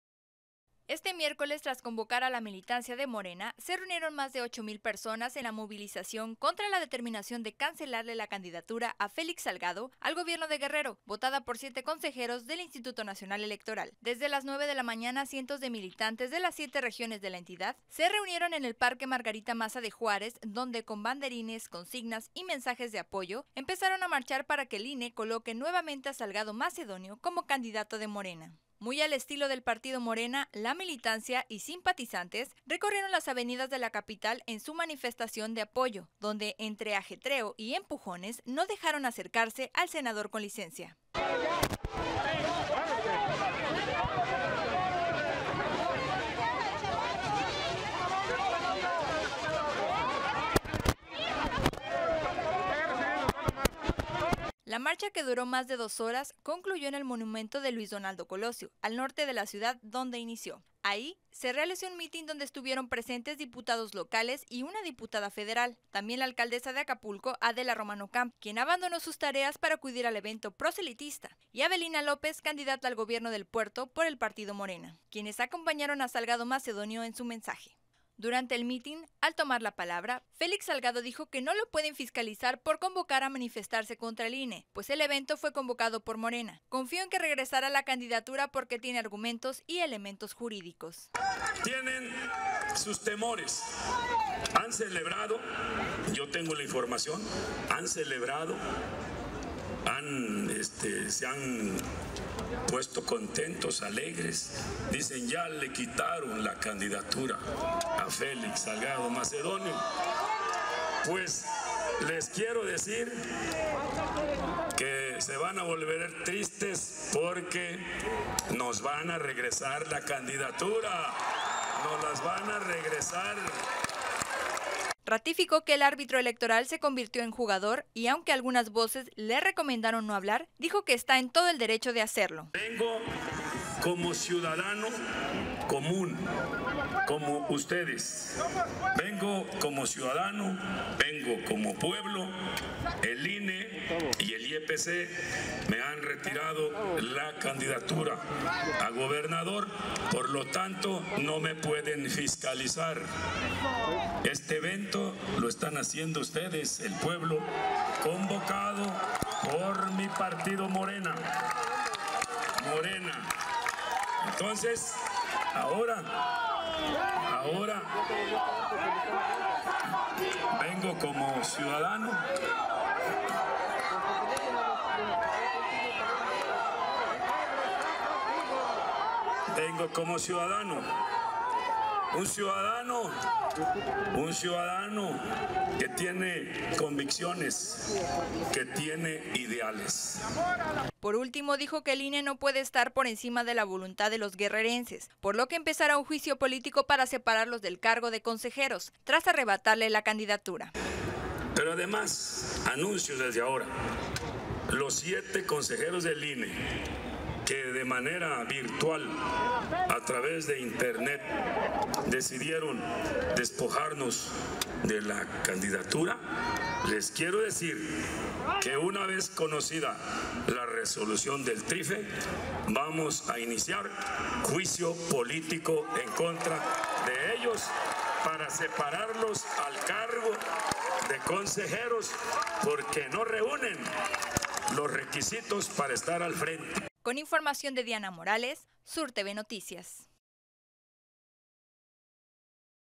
Convoca Félix Salgado a marcha contra el INE, se reúnen miles de todo el Estado en Chilpancingo
En su mensaje, Félix Salgado advirtió al INE que no se van a dejar, “en Guerrero no nos rajamos, y no hay paso atrás, que sepan las autoridades que no nos vamos a dejar”, añade.